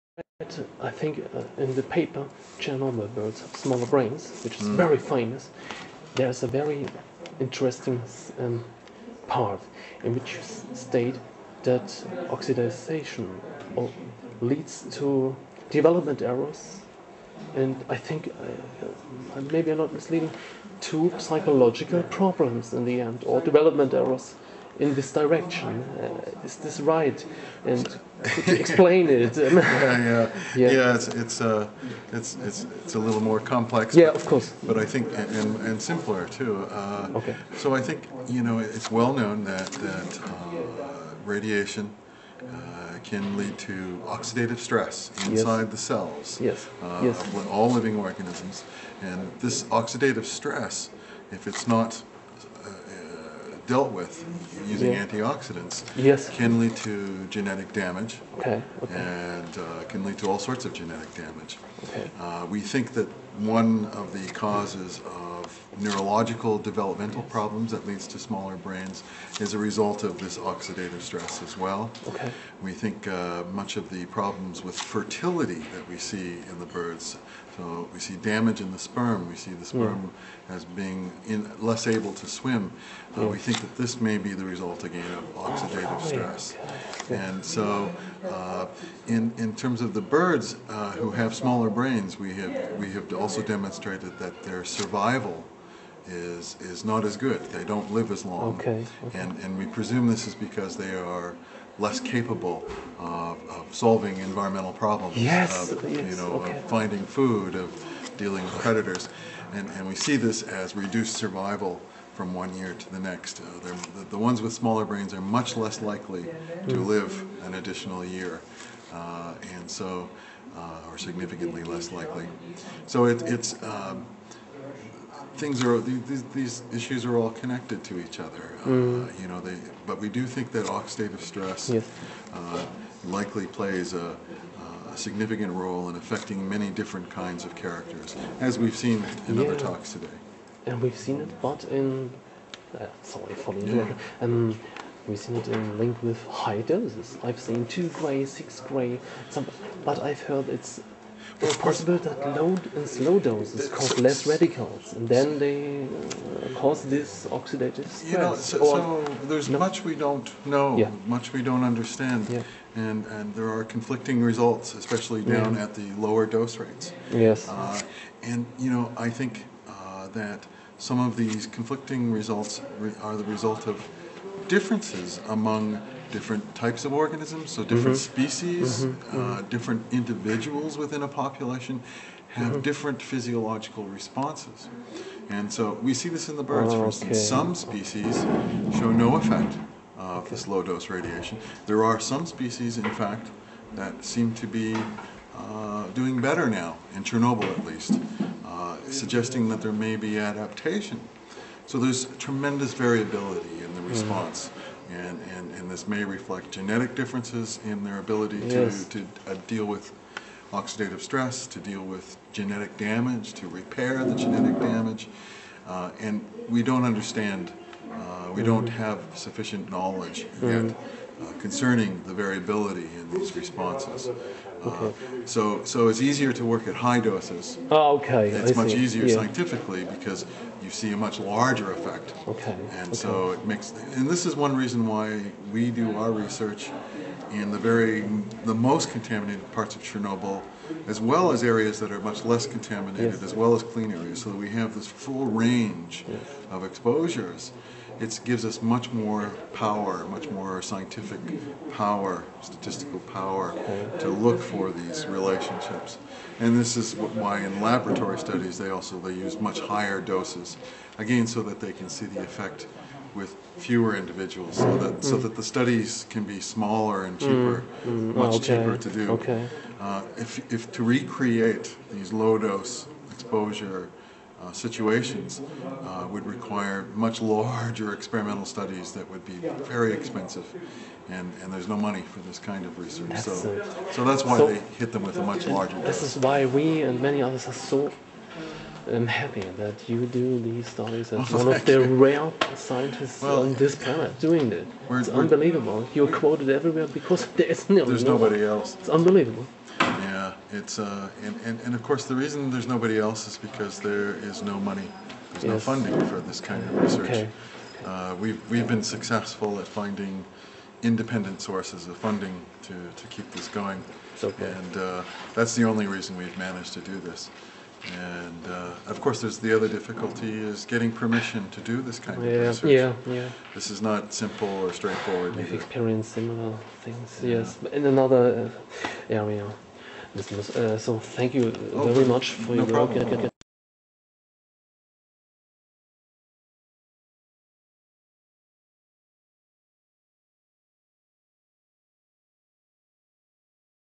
Professor Timothy Mousseau at Fukushima Symposium (03/042014)
Professor Timothy Mousseau at Fukushima Symposium on March 5th 2014. More abstracts He is the author of many Chernobyl studies, among them: Chernobyl birds have smaller brains